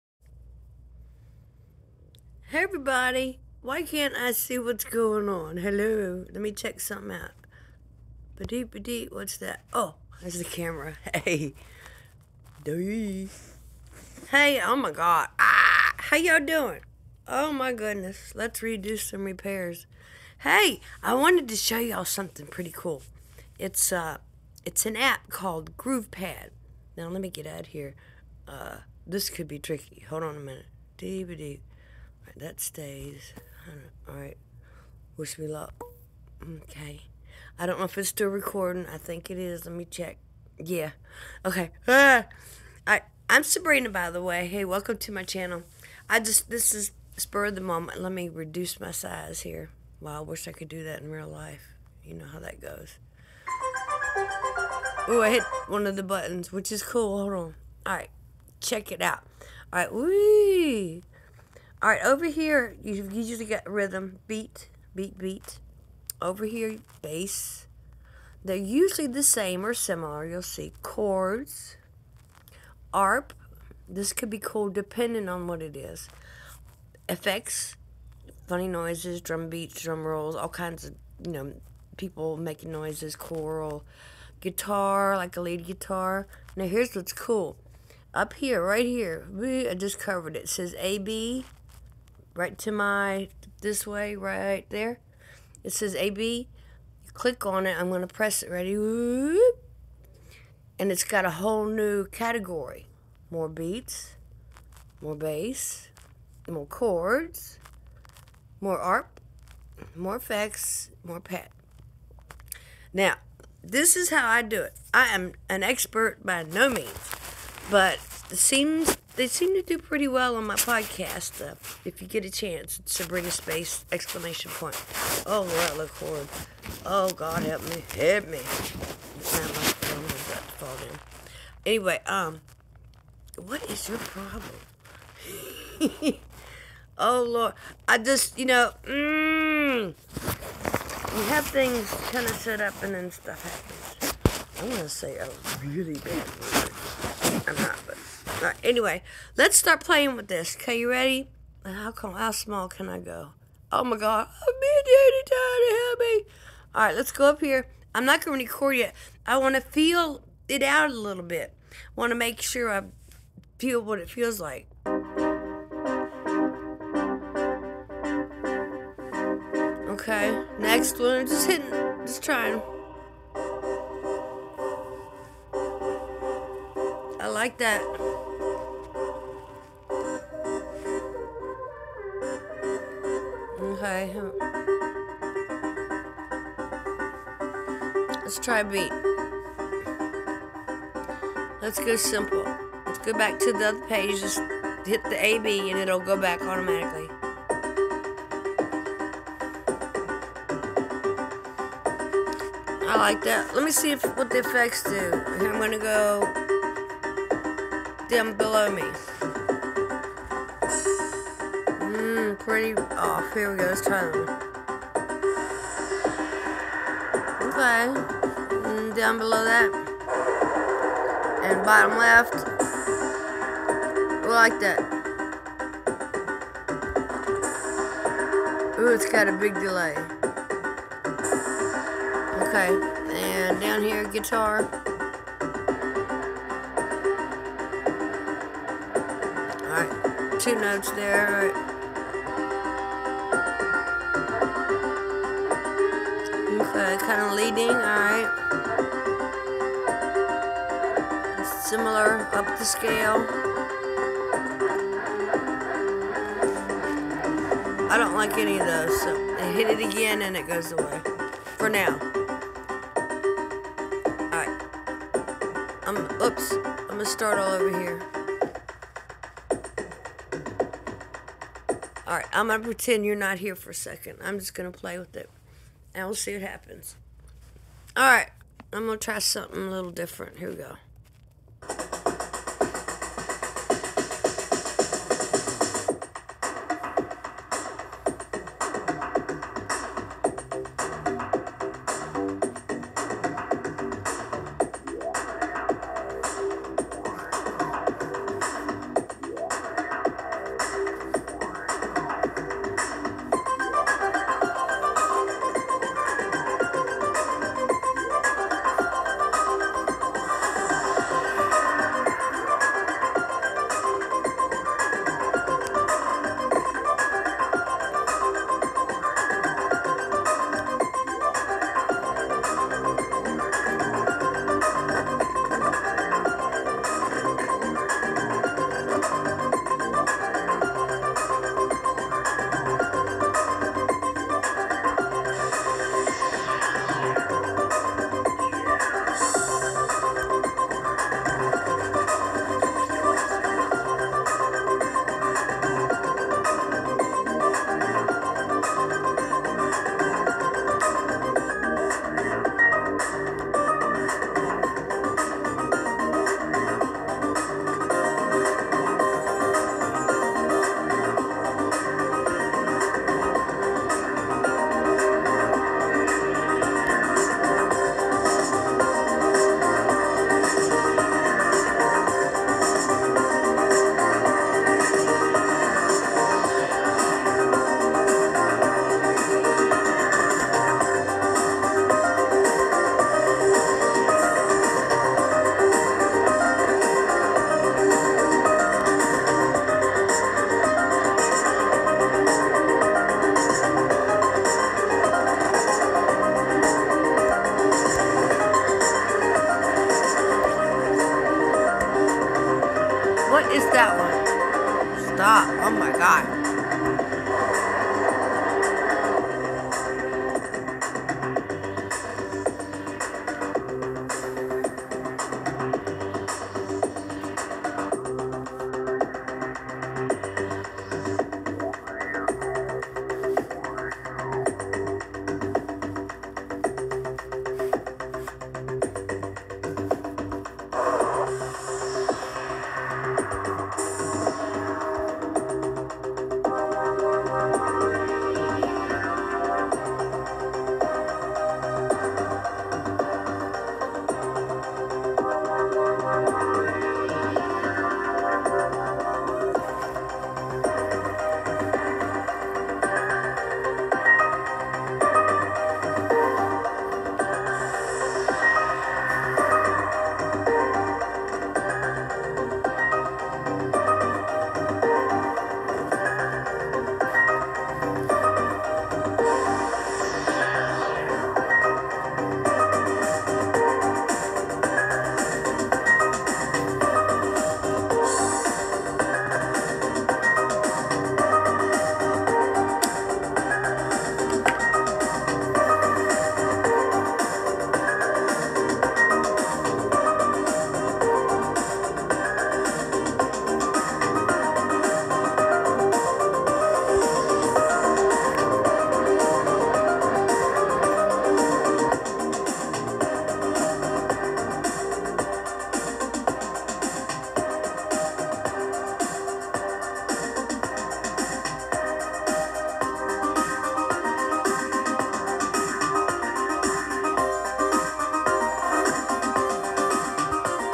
Groovepad App Explained. Beginner Fun. How to play.
Welcome to GroovePad, it's a great app and it's very fun especially for beginners. here's a short video example of the things you can do with it, of course, this was just practice and not ready for publishing or putting on my podcast, etc. but, it is so much fun! learn your rhythms, bass,chords, drums, melody, guitar, arp,effects and more. Enjoy the short tutorial!  🙂❤️🎹